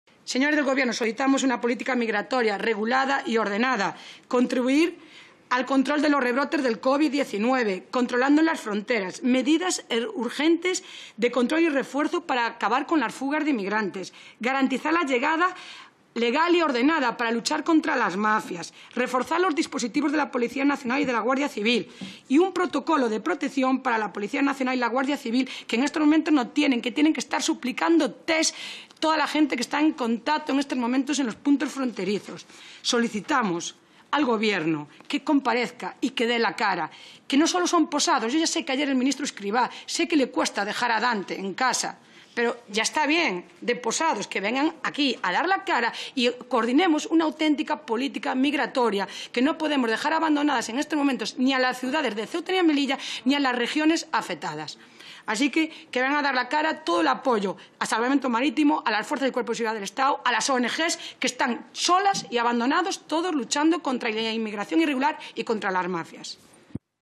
Vázquez pide "no abandonar a Ceuta y Melilla ni a las regiones afectadas" por la política migra
La portavoz del PP en materia de inmigración, Ana Vázquez, señala que "ya está bien de posados",  en referencia a una entrevista concedida por el ministro a los medios, antes de exigir al Gobierno "una auténtica política migratoria" y "no abandonar a Ceuta y Melilla ni a las regiones afectadas" por este tema.

(Fuente: Congreso)